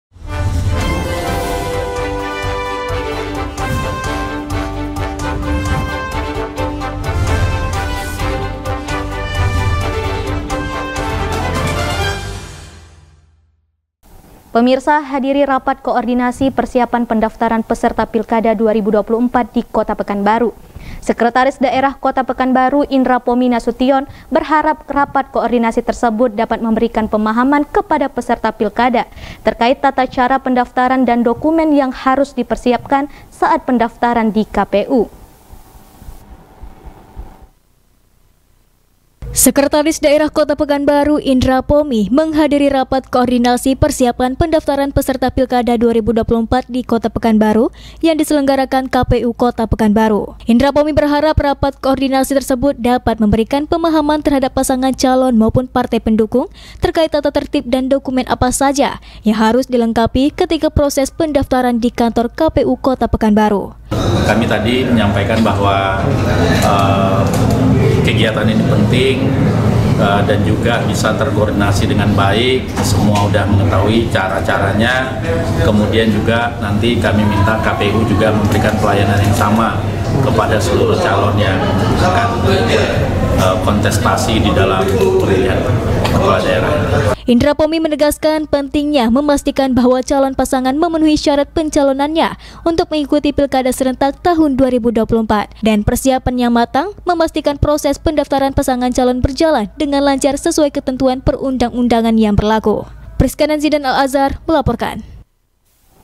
SEKDAKO PEKANBARU HADIRI RAKOR PERSIAPAN PENDAFTARAN PESERTA PILKADA
Pekanbaru, riautelevisi.com- Hadiri rapat koordinasi persiapan pendaftaran peserta PILKADA 2024 di Kota Pekanbaru, Sekretaris Daerah Kota Pekanbaru, Indra Pomi berharap Rakor tersebut dapat  memberikan pemahaman kepada peserta PILKADA terkait tata cara pendaftaran dan dokumen yang harus dipersiapkan saat pendaftaran di KPU.